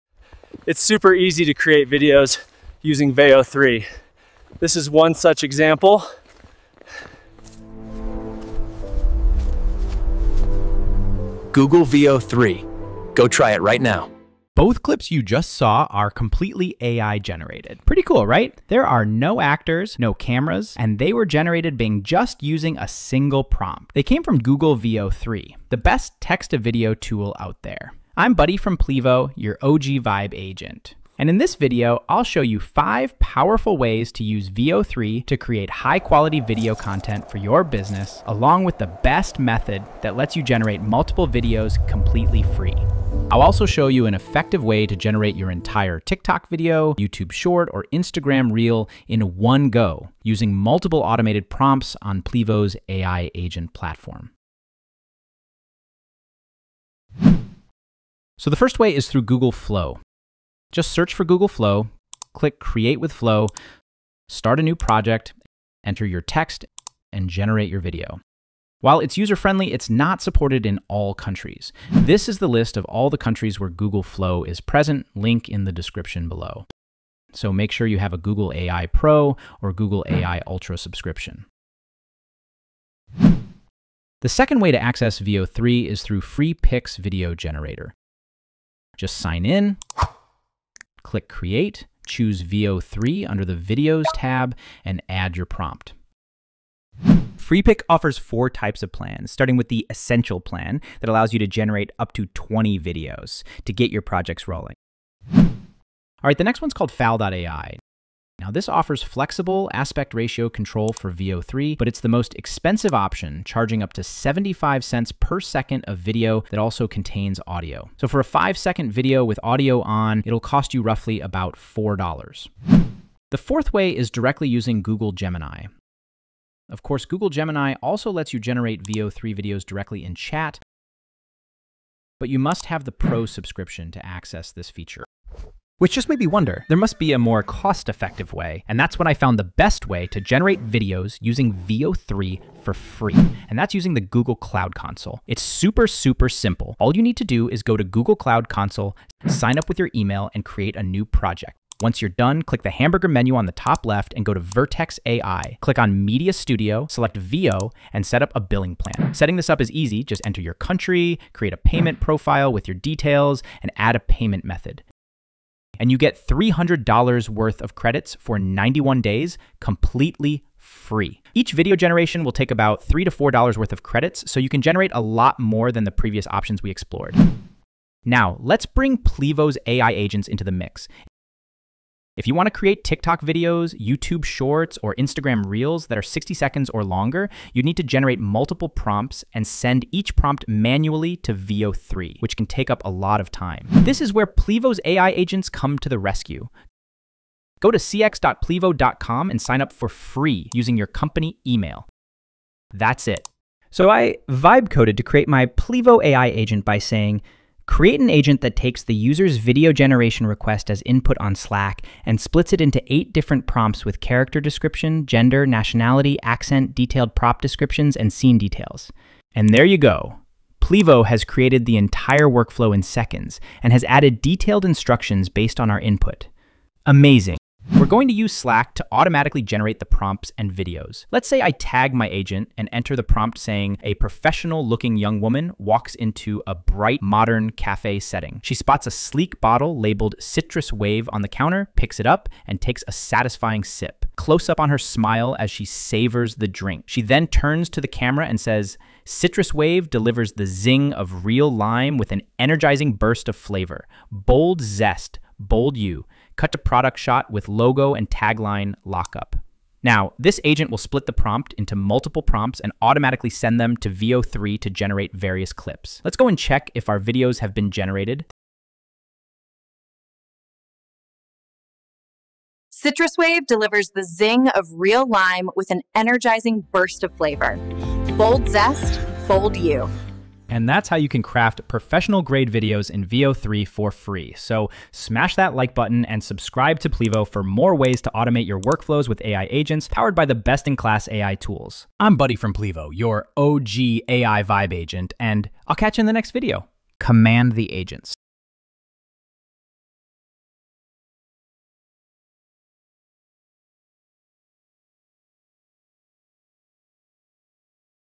How to make Viral AI videos for Tiktok and Instagram using Veo 3
In this video, we’ll show you how to create viral AI videos for TikTok, Youtube and Instagram using Google Veo 3 and Plivo’s AI Agents. We’ll guide you through 5 powerful ways to access Veo 3 and automate your entire social media content creation process. You can generate stunning, realistic-looking videos by just using a single prompt! We’ll also show you how to create your own AI Agent for social media, automating video generation through Plivo’s platform.

We talk about how Plivo’s AI Agents automate video creation through AI social media automation. You can easily integrate Slack to generate multiple prompts and automate the entire video creation process with Google AI Veo 3.
Whether you want to create TikTok videos, YouTube Shorts, or Instagram Reels, we’ll cover everything you need to get started and boost your social media engagement.

Timestamps:

0:00 - Intro
0:59 - Google Flow
1:26 - Freepik
1:48 - Fal.ai
2:05 - Google Gemini
2:25 - Google Cloud Console
3:00 - Plivo's AI Agents for one-prompt video generation